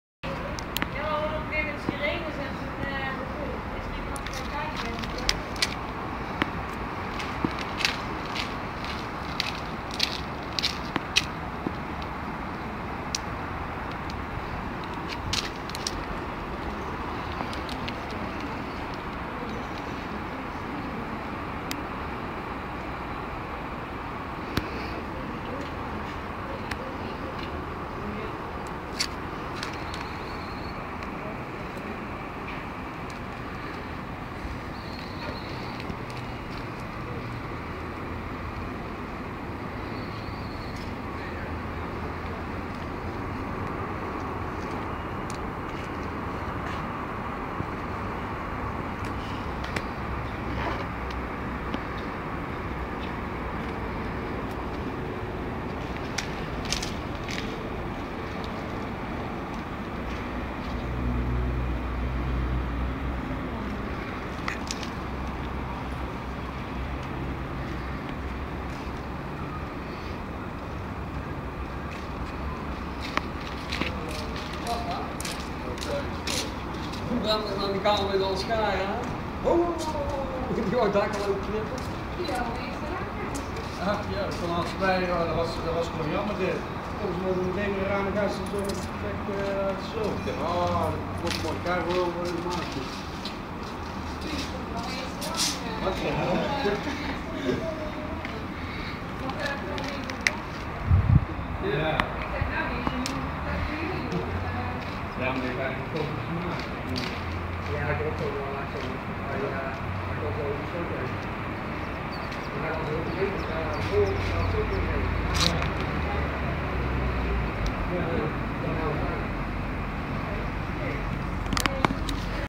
a1 20-343 rapidresponder 20-143 prio1 6733 6880 kleine vko
hier un filmpje van een kleine vko was niemand meegenomen naar het ziekenhuis